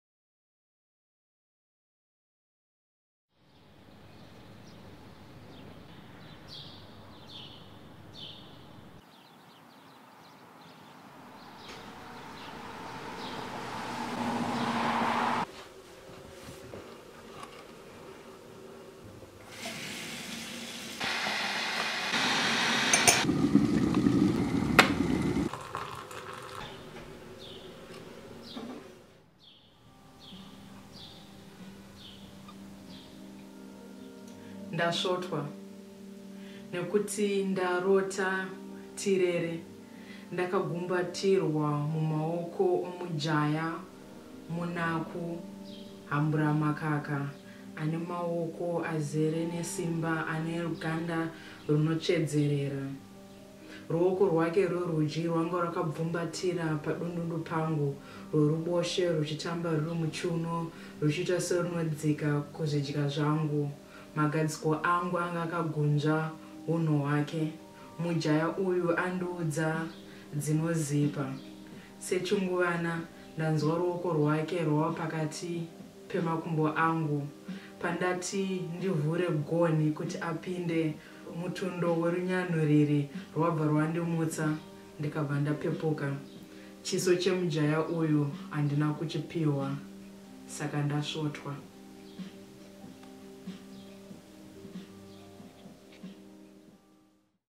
Linda Gabriel - Ndasvotwa
Check out this new video of the poem 'Ndasvotwa' by the brilliant poet Linda Gabriel.
Linda, born in 1985, is one of the crop of exciting female performance poets to emerge out of Zimbabwe. Bilingual, her linguistic versatility is forged into brilliant poems written in either English or Shona, depending on which language effectively expresses what issue. The 30year-old powerful orator has been hooked on performance poetry since high school in 2001 and has since participated in major literary events in Zimbabwe, the rest of southern Africa and Europe. Mostly her work celebrates womanhood and its challenges. She uses the stage to tell untold stories and address taboo matters.

If you don't speak Shona and should therefore experience troubles understanding this poem, this could be an interesting starting point for you